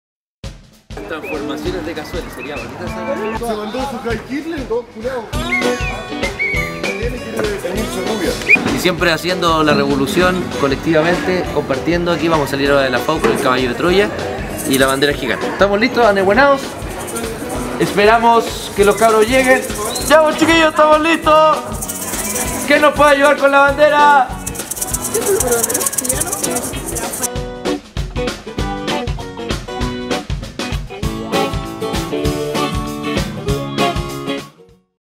La Ruta de Cazueli: Teaser Capítulo 10
La ruta de Cazueli, partiendo desde la FAU con el caballo de Troya, llevando la bandera gigante de la educación y la alegría a la marcha.

Se viene junto a FAUNA.